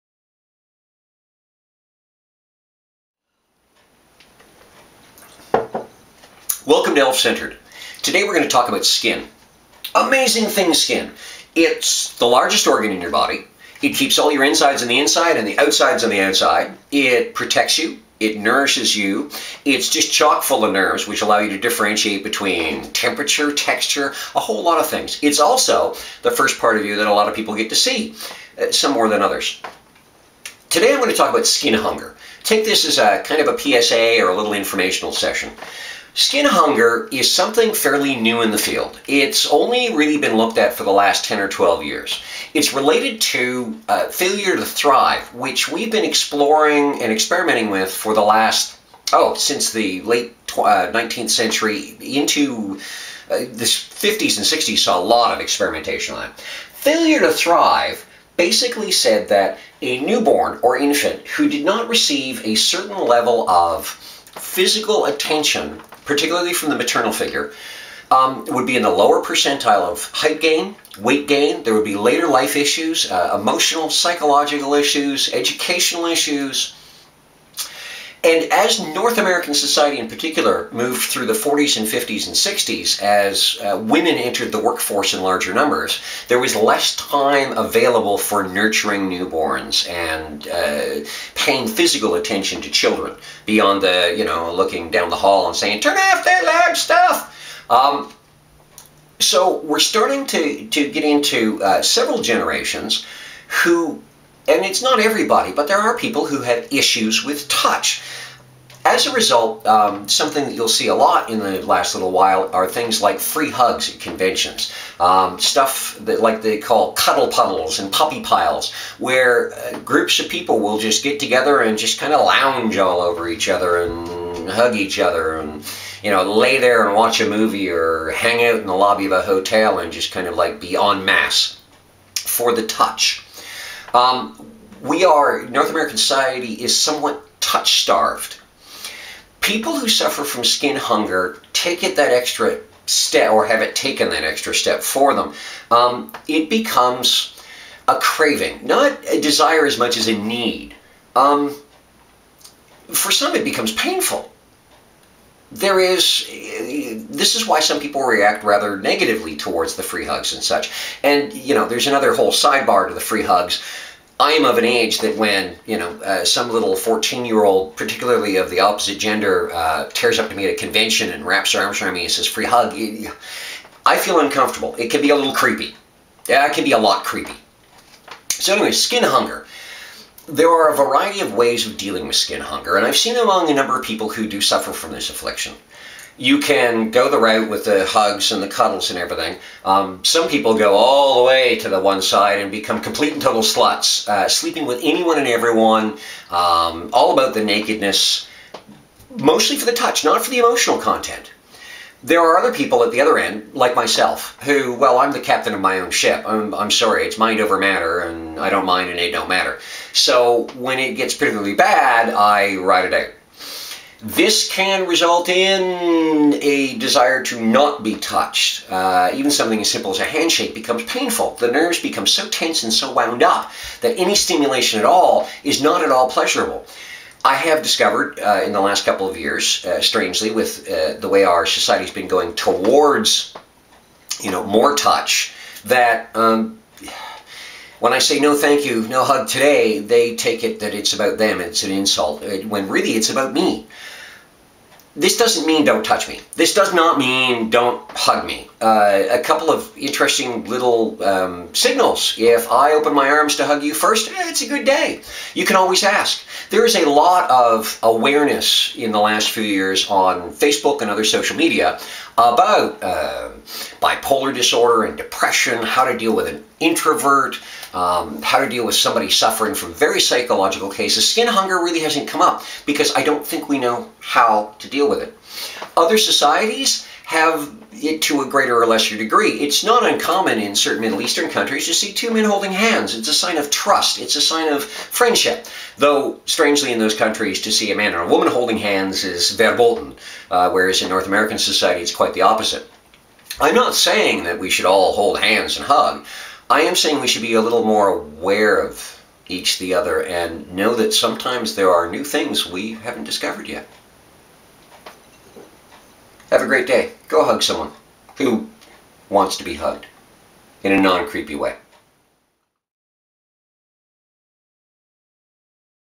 Elf-Centered - Bodytalk: Skin Hunger
A short PSA or informational video discussing Skin Hunger.
Don't know what that is?